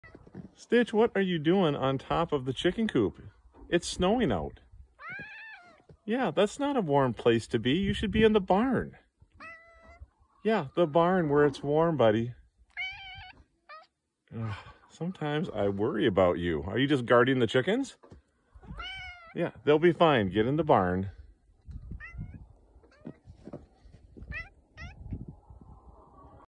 Talking to my barn cat in a snowstorm
Stitch our farm cat loves to have conversations with me, and sometimes argues with me too!